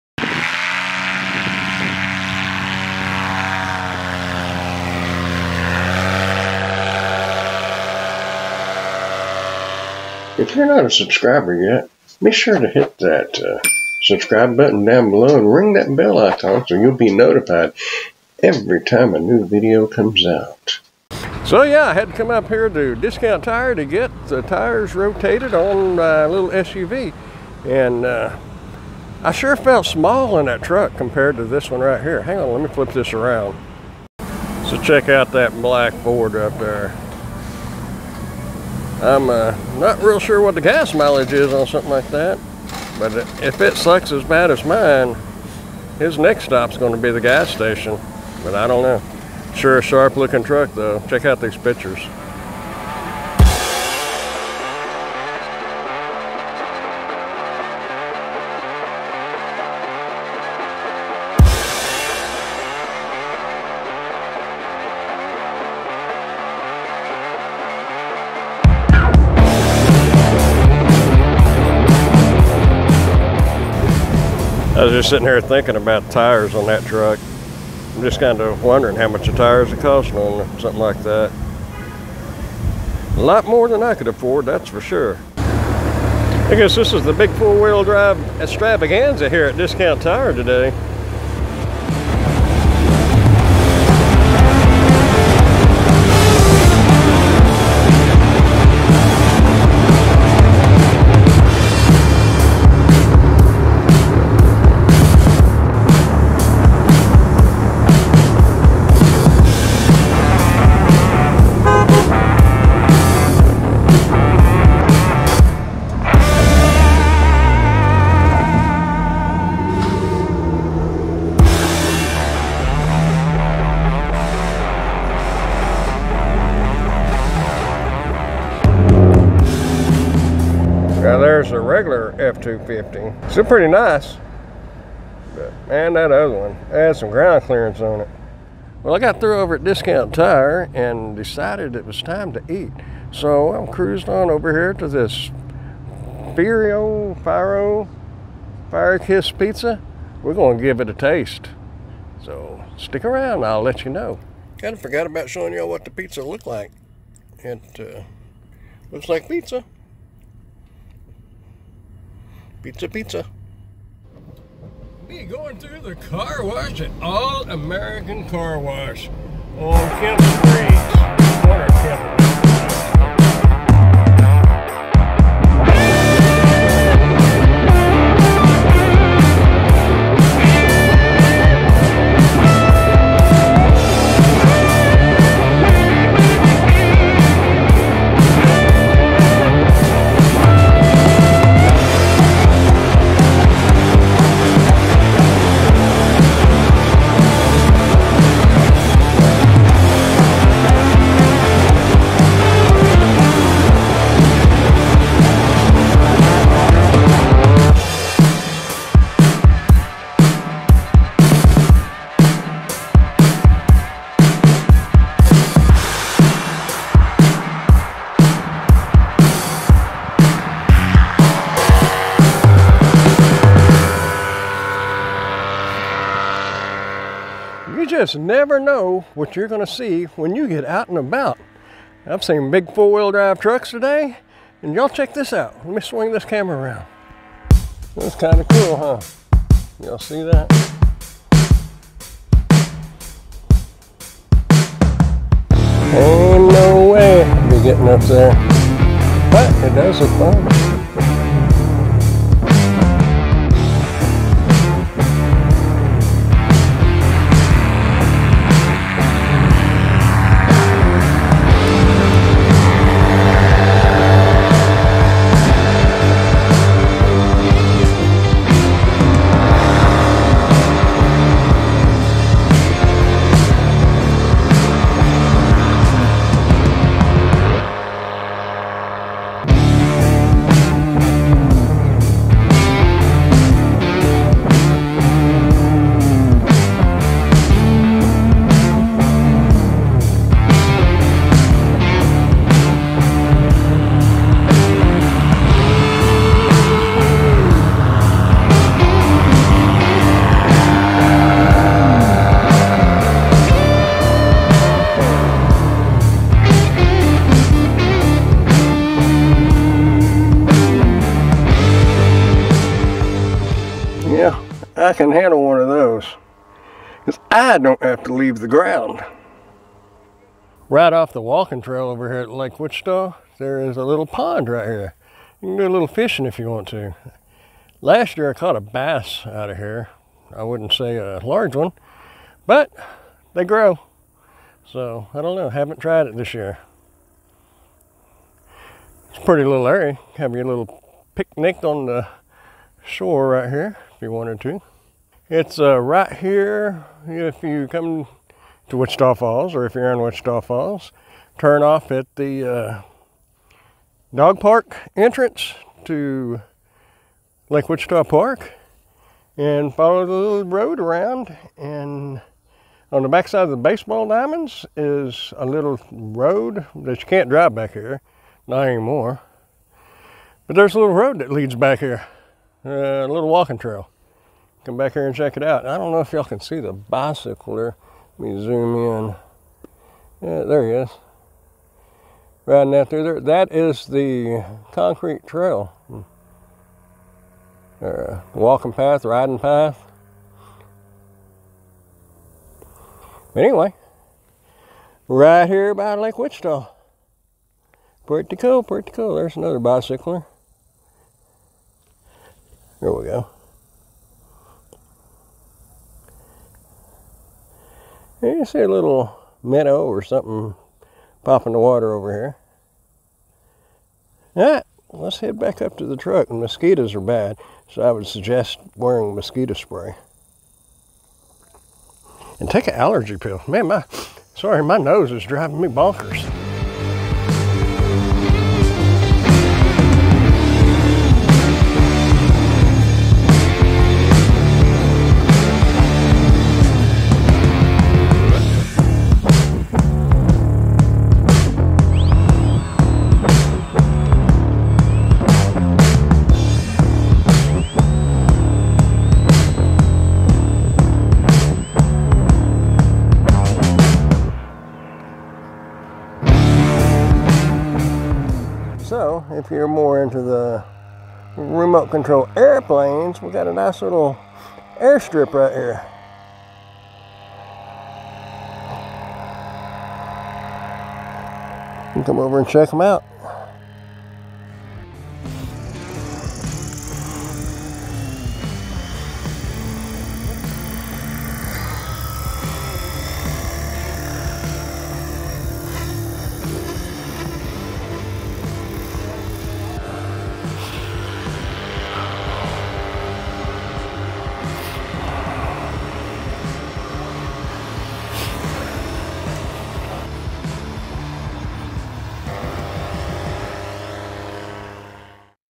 Riding High Texas Style | Wichita Falls Texas
Riding high Texas style in Wichita Falls Texas!
Ride along and check out 4x4 Fords (@that_6.7_villain),Firo Fire Kissed Pizza, fly high on a ultralight and then we’ll check out some radio controlled aircraft at Lake Wichita Park.